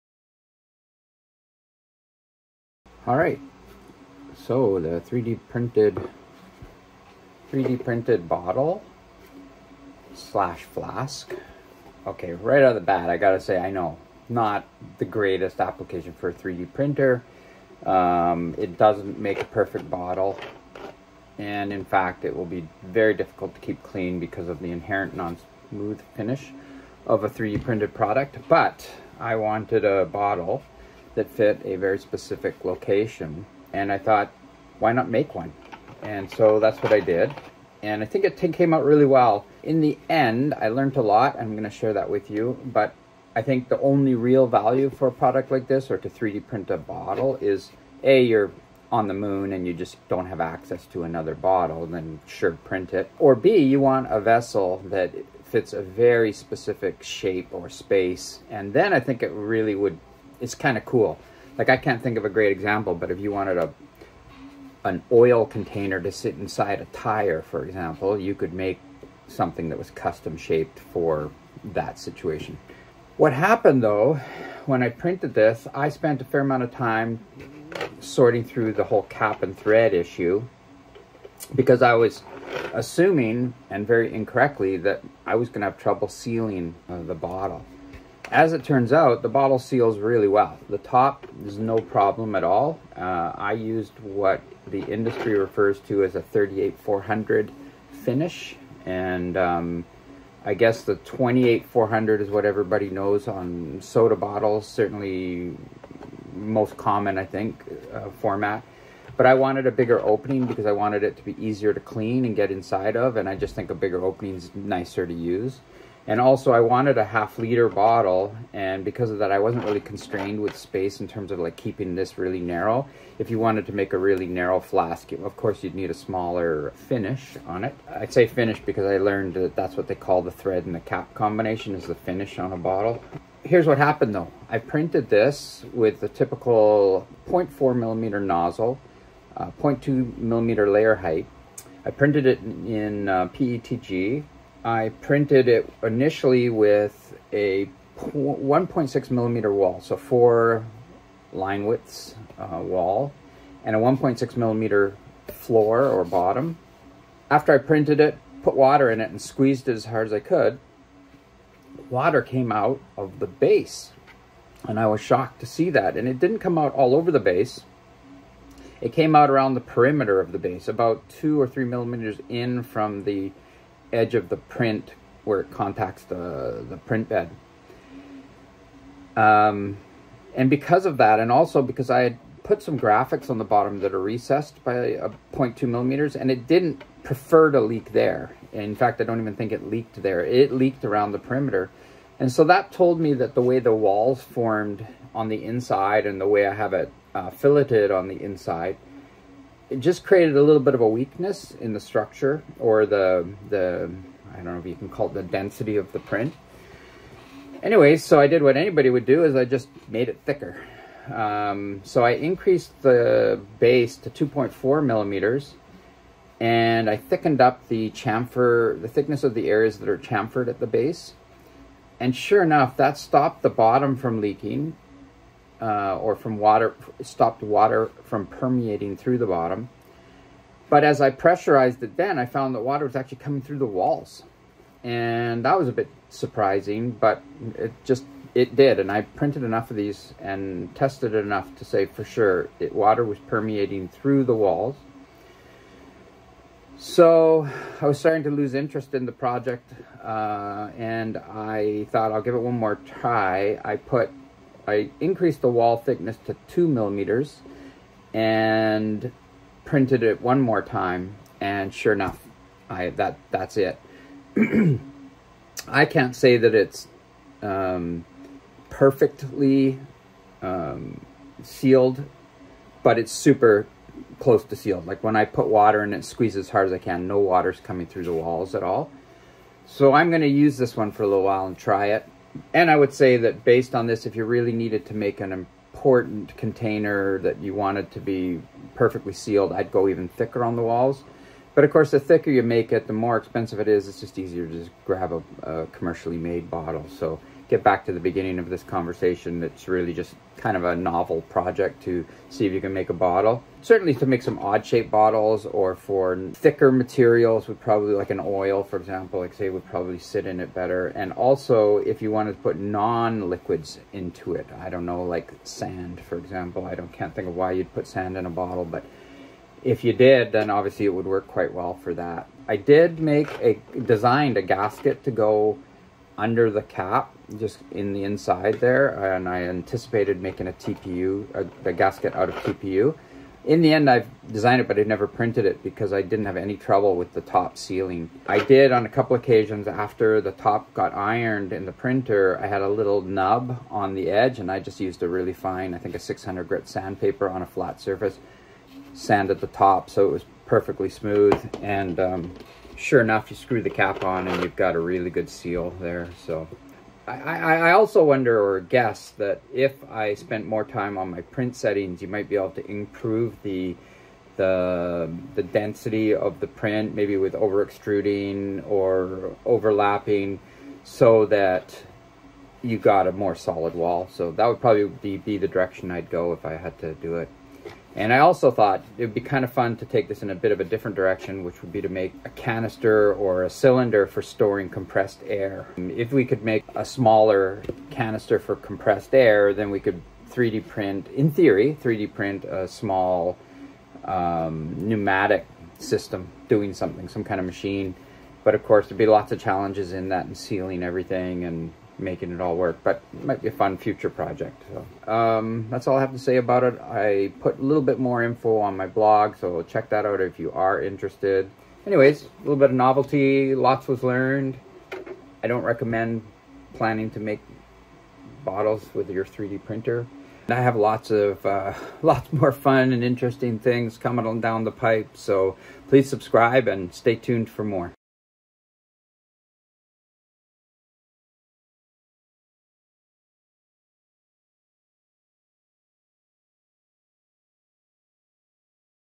3d printed 500ml flask
I designed and 3d printed a flask to learn that I probably shouldn't.  it works but there are some caveats to that.